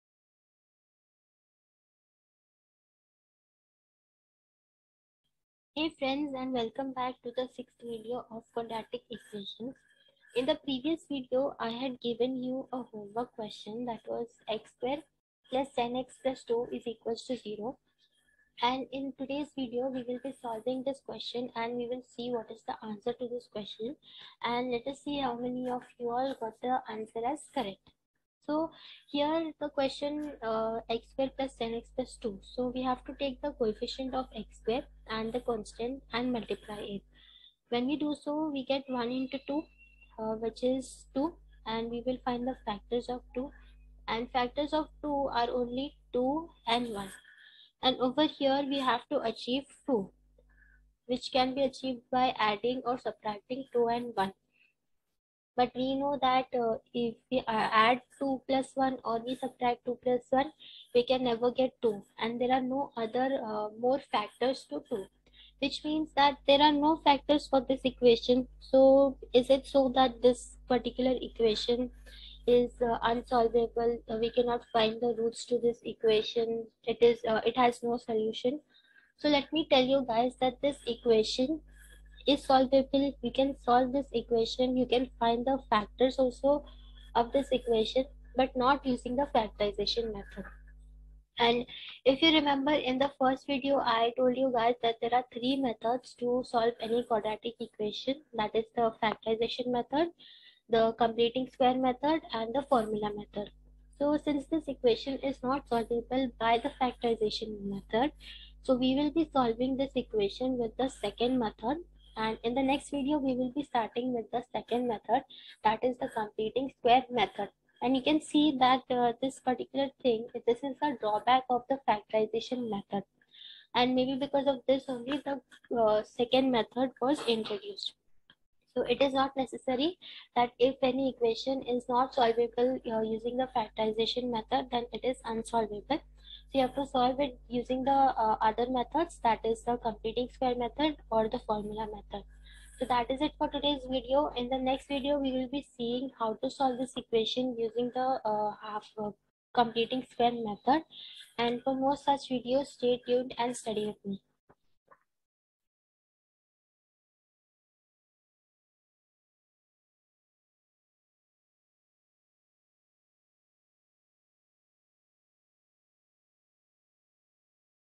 Quadratic Equations|Factorisation Method| x²+10x+2=0
Hello everyone, so today we will learn how to solve Quadratic equations using the Factorization method . In this video, we will see the solution of the homework question given in the previous video. I hope you all understood the concepts discussed in the video.